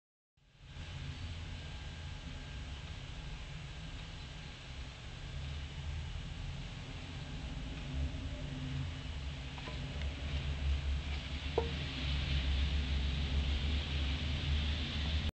Cube 2
This is a view from my apartment at the Museumsquartier in Vienna Austria